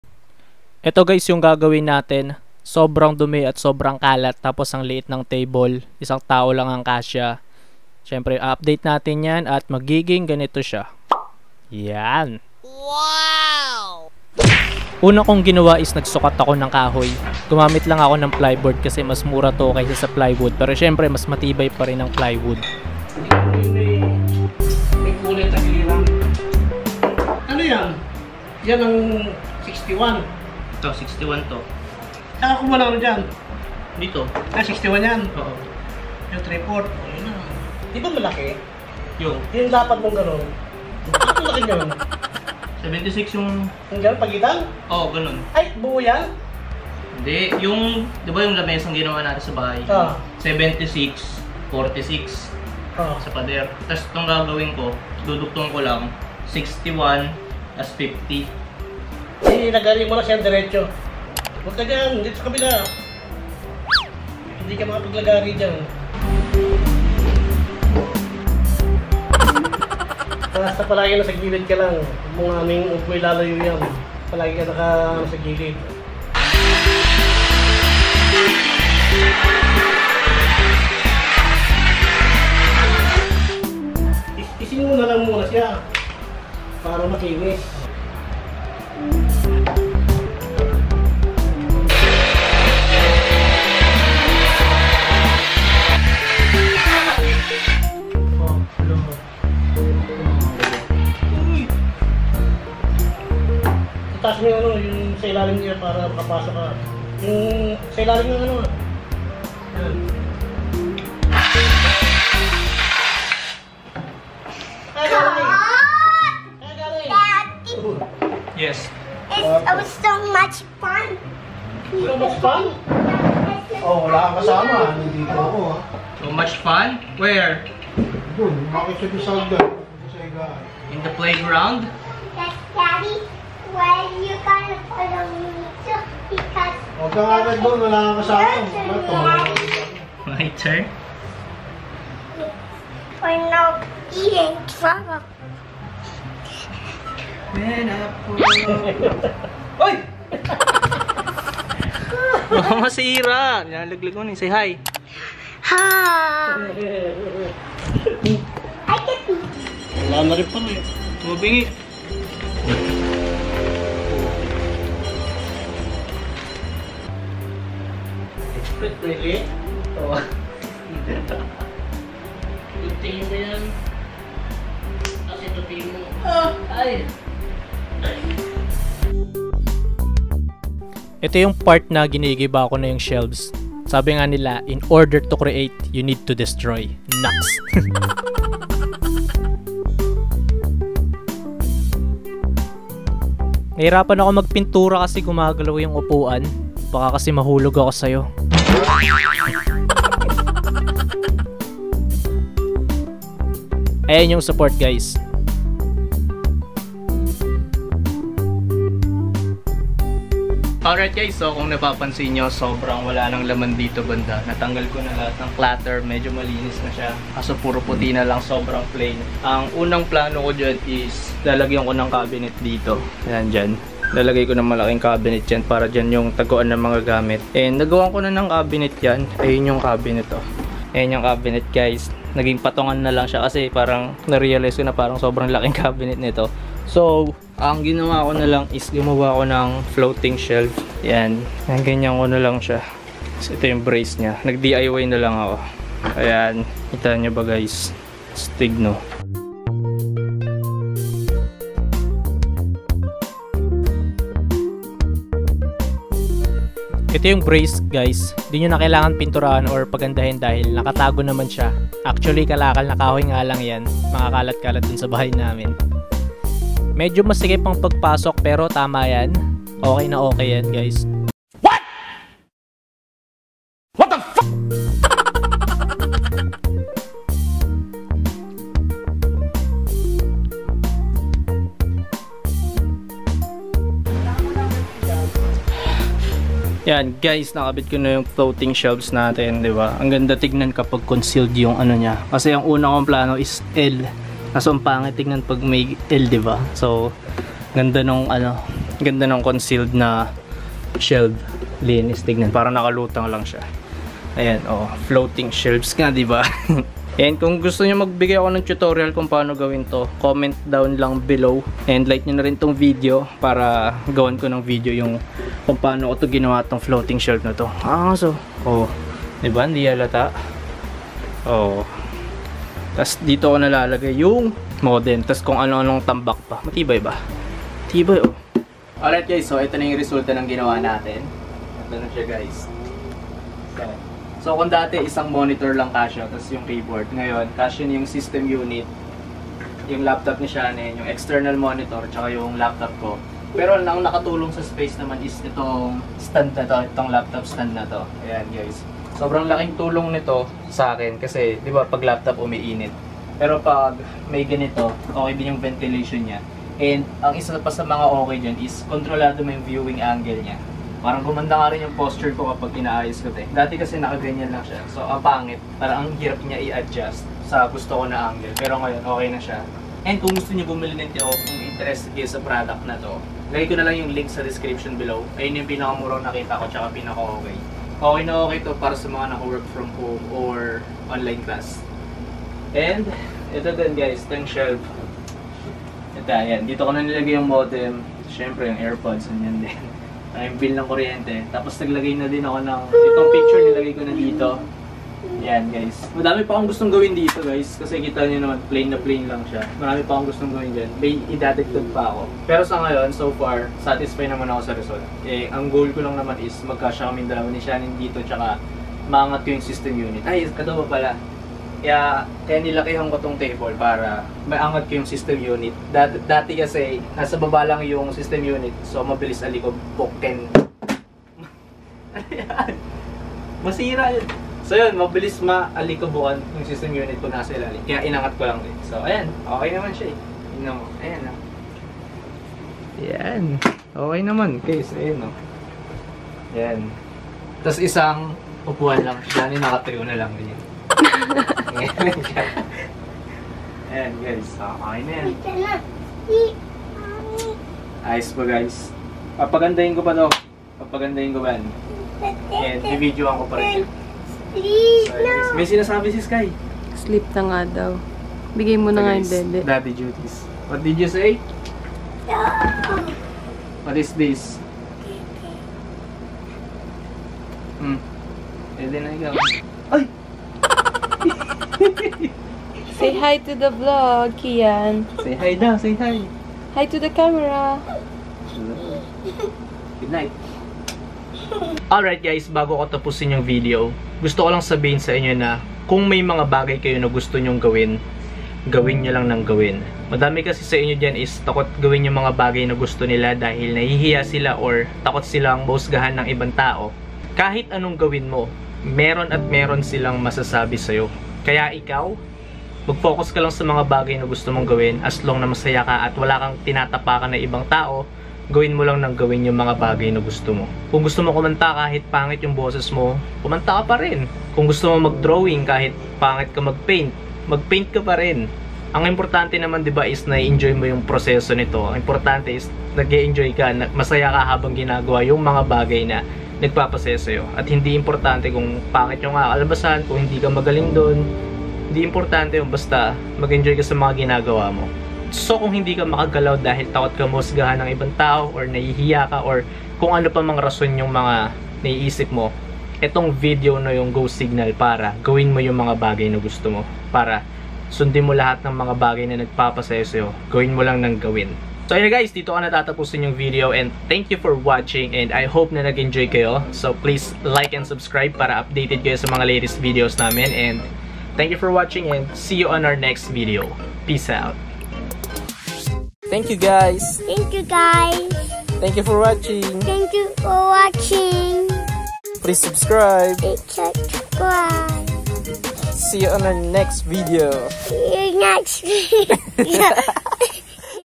How to create DIY Home Office Desk- Work From Home Setup Room Makeover + Floating Shelve -KSFamilyTV
Hi Guys!

Share ko lang yung ginawa kong Room makeover para sa Workfrom home setup ko. Diy PC Table or Computer Table plus DIY floating Shelves.

Ignore tags.
Paano gumawa
How to Create
Desk setup
DIY Desk PC
Computer Table
PC Table